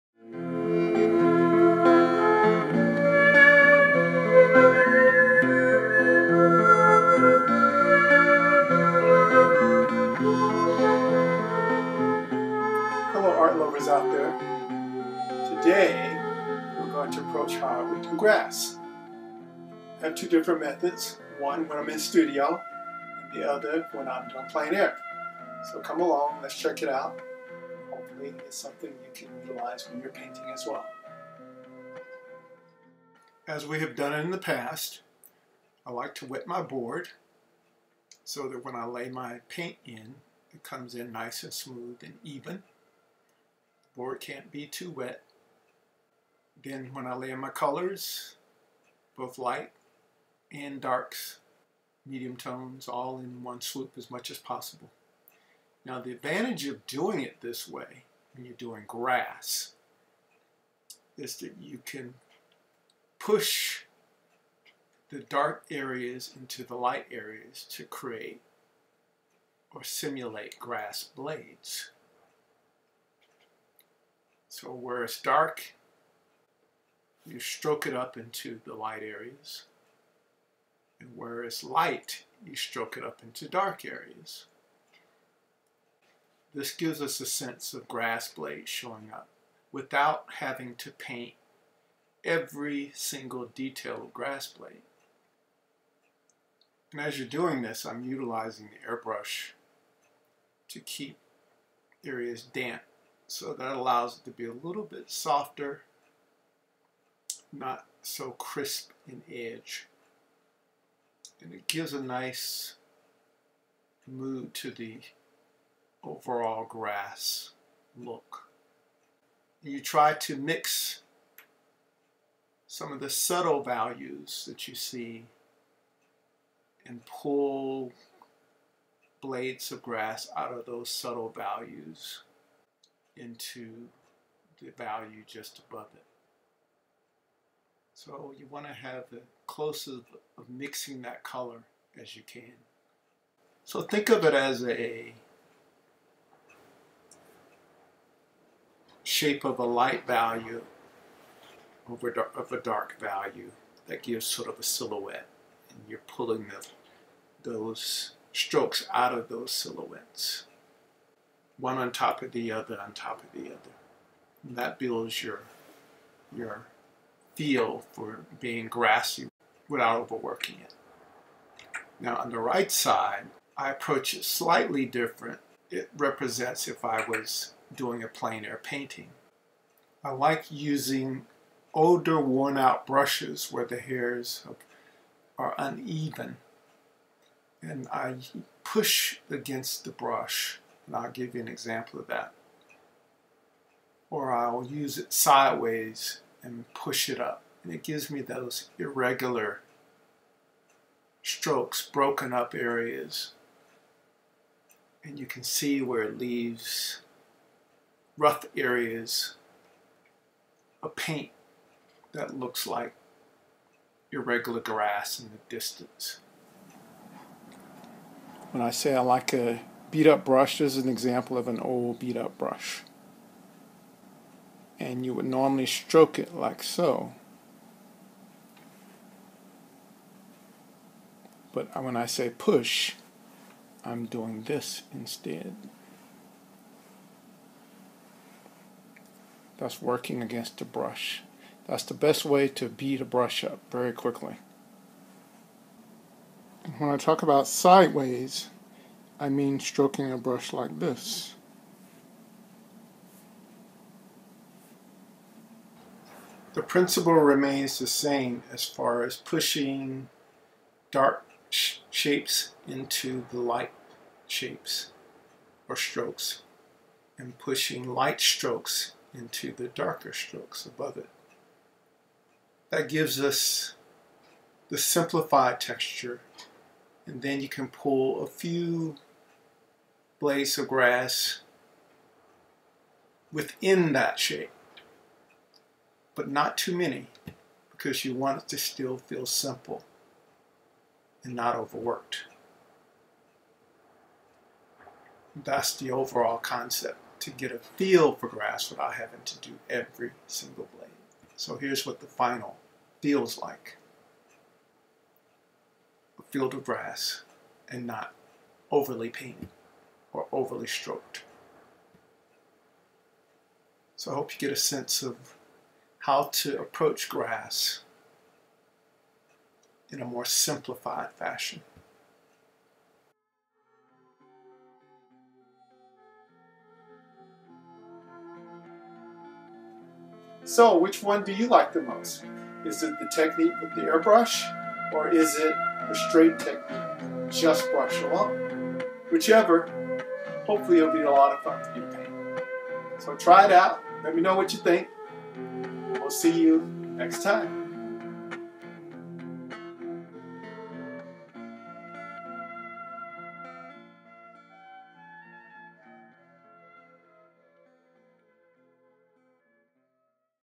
Artist Technique Depicting Grass In a Painting
This is a demo on how to depict grass in a painting.